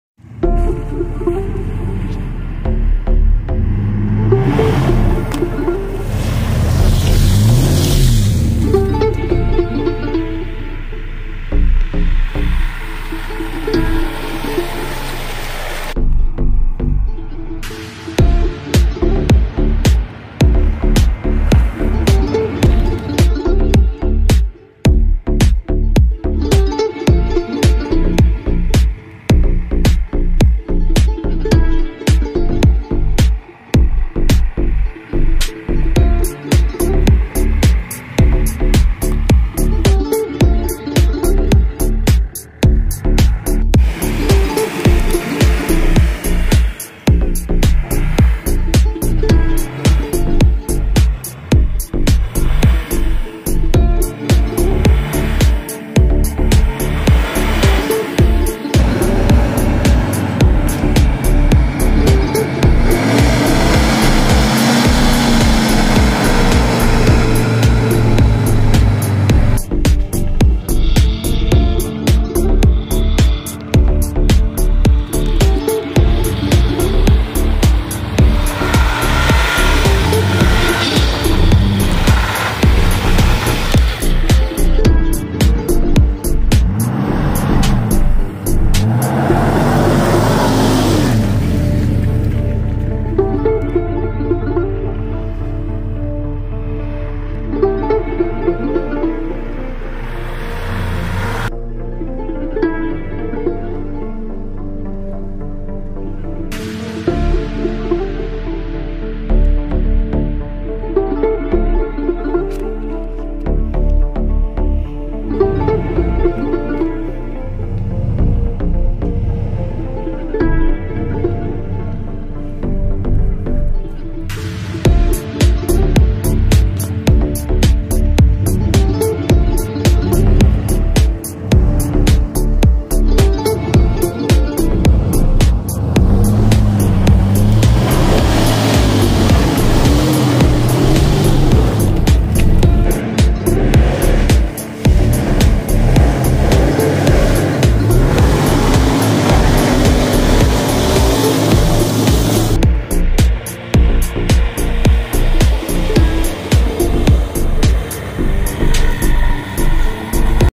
Nissan Patrol Y61 BMW m57 3,0 d & Jeep Grand Cherokee WJ 4,7 V8 - wspólnie w terenie OFFROAD
Nissan patrol jeep WJ
BMW m57
4,7 V8